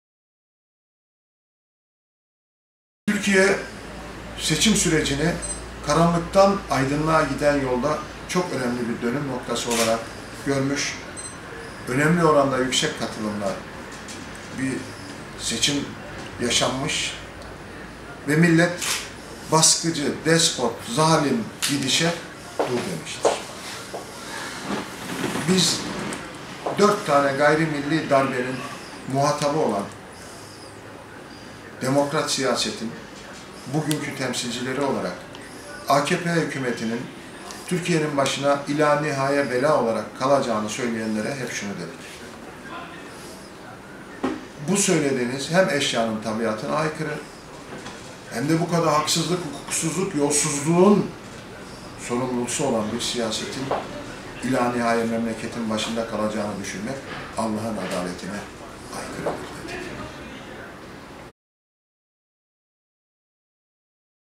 ZULMÜN İSTİKRARI BOZULACAK
DP Genel İdare Kurulu Üyesi Erdem Demirdağ, "Hep istikrar bozulacak diyorlar. Türkiye'de artık zulmün, baskının, haksızlığın, hukuksuzluğun istikrarı bozulacaktır" dedi.